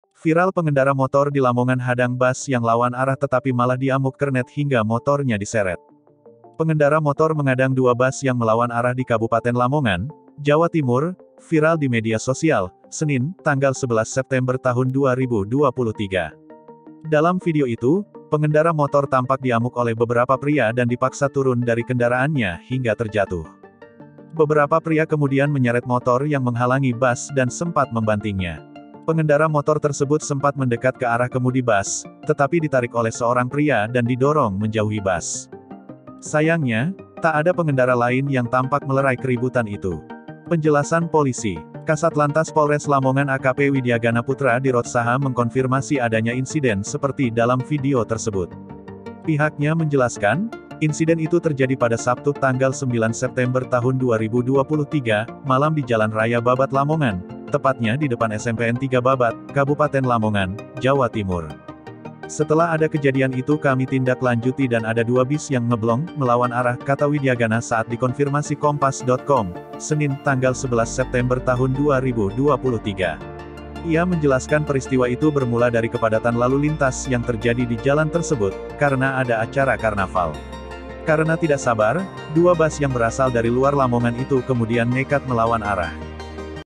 Viral Pengendara Motor di Lamongan Hadang Bus yang Lawan Arah Tetapi Malah Diamuk Kernet
Viral Pengendara Motor di Lamongan Hadang Bus yang Lawan Arah Tetapi Malah Diamuk Kernet hingga Motornya Diseret.